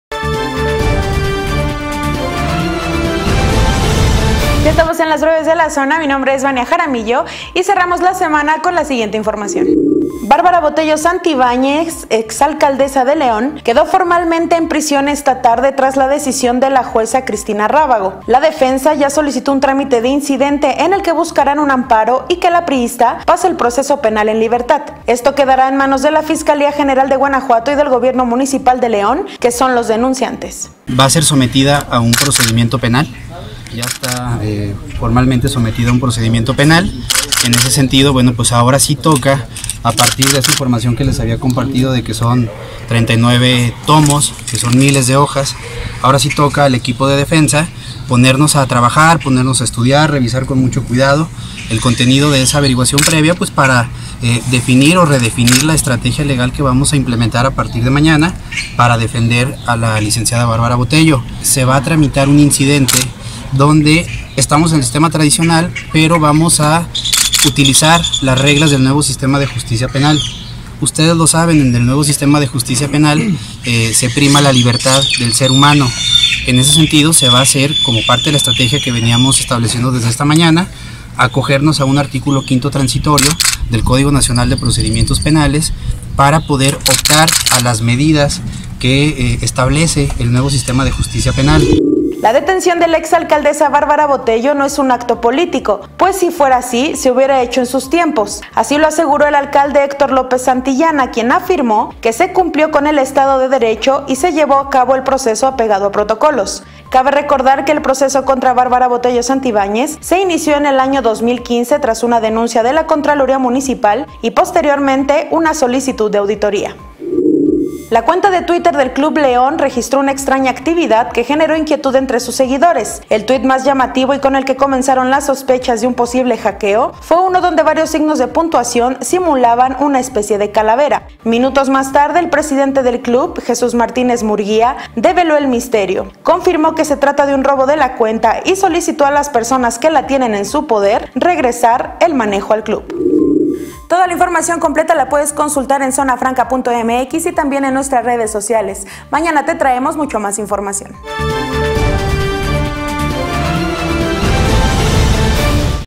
Breves de la Zona 31 de mayo 2019 emisión nocturna
Hoy en las BrevesDeLaZona💻📰 con Vania Jaramillo
- León: Dictan auto de formal prisión a BárbaraBotello por presunto delito de peculado.
- Si la detención de Bárbara fuera política, la hubiéramos hecho en tiempo: López Santillana.
- IsmaelSosa llega al ClubLeón entre “Hackeo” a la cuenta oficial del club en twitter.